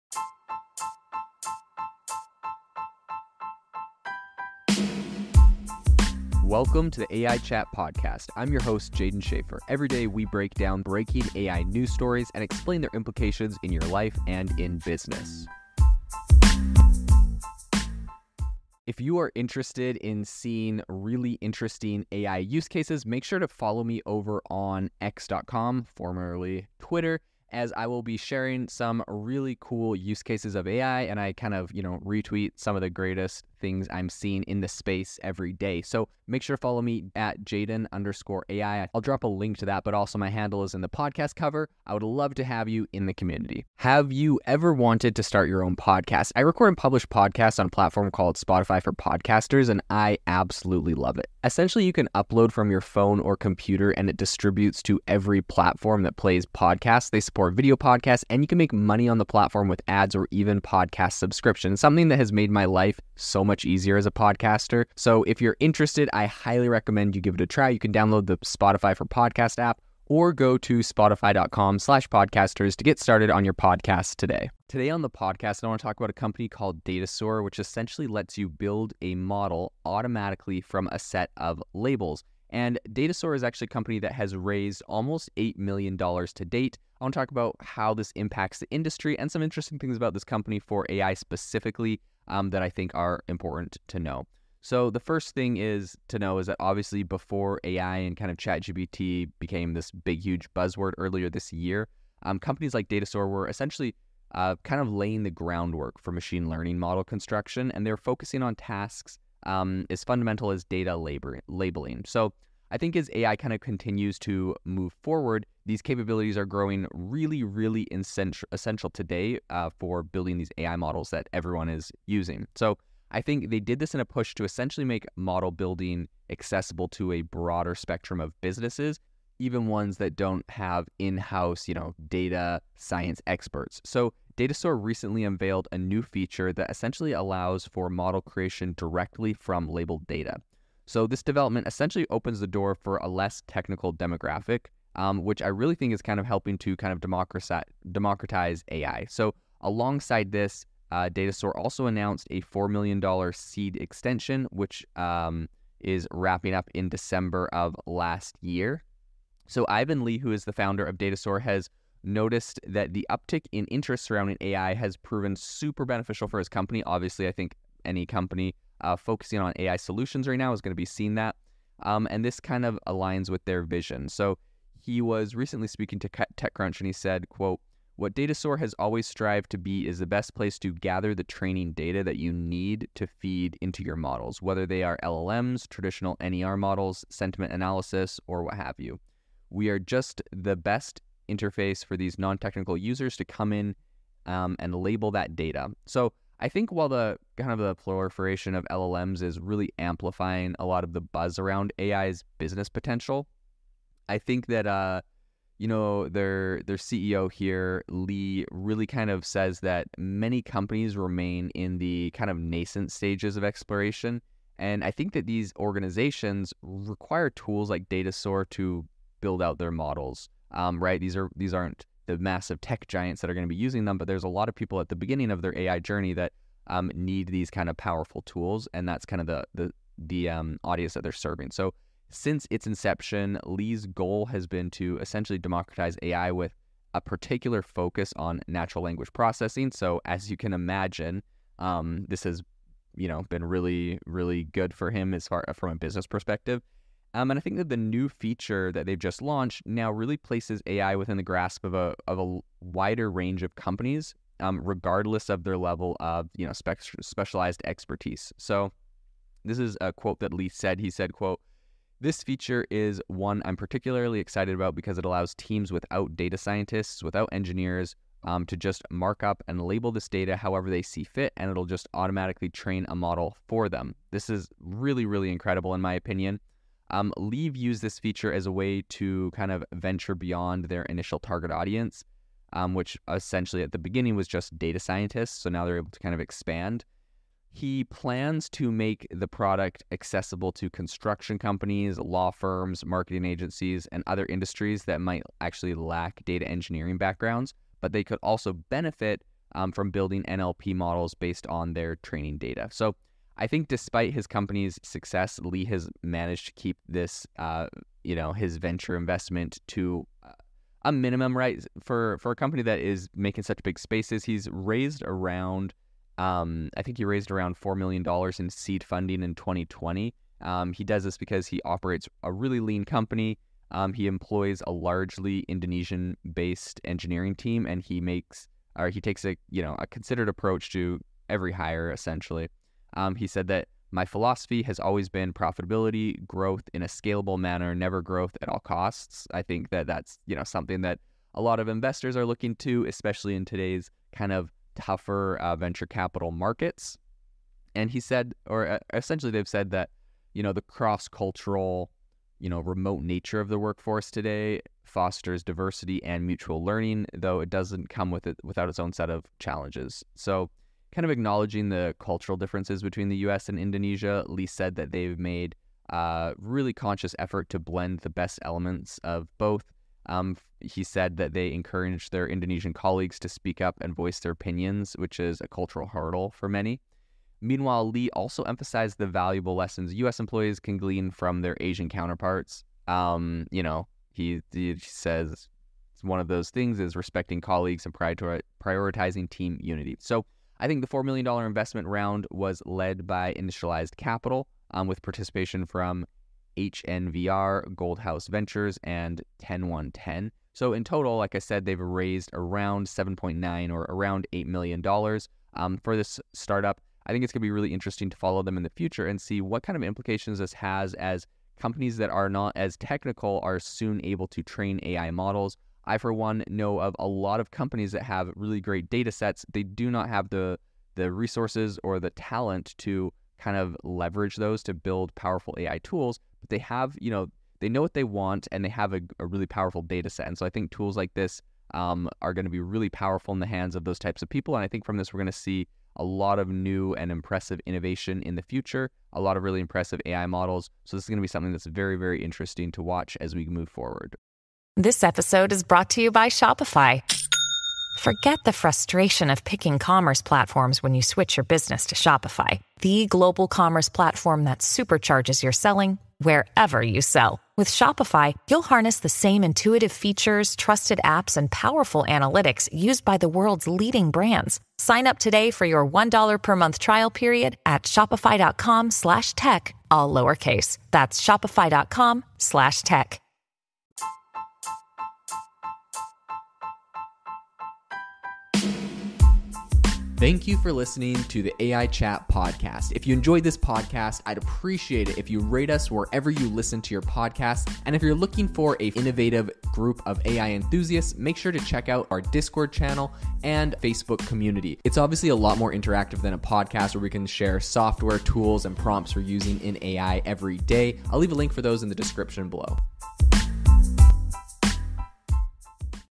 Datasaur Raises $7.9M to Let You Automatically Build AI Models From a Set of Labels
In this insightful episode, we delve into Datasaur's groundbreaking AI feature that enables users to auto-generate machine learning models using pre-defined labels, revolutionizing data annotation processes. Learn how this tool simplifies AI training, drastically reduces the time and expertise needed, and discover its potential applications in various industries.